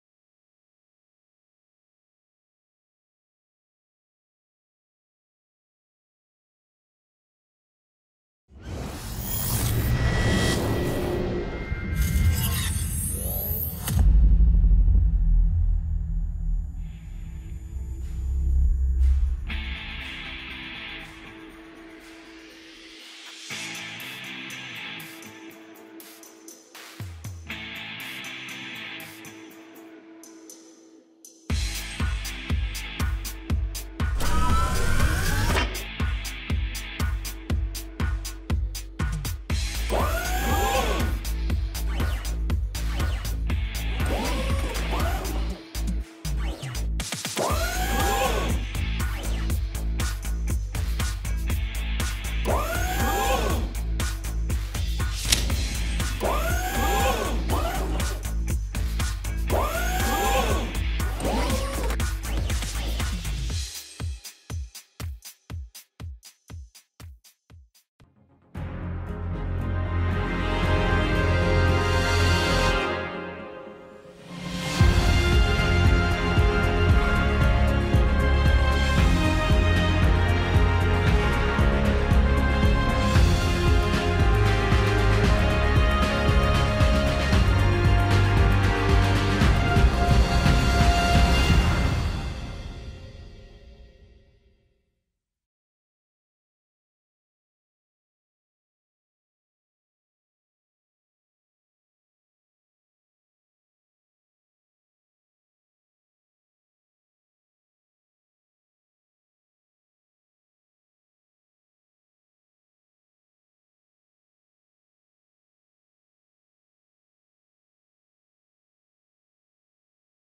Wars Gear C - Vancouver Film School (VFS)
Dare to fight this war machine! 👊 Clark Liu impressed us with this amazing clip, created through our Foundation Visual Art & Design program.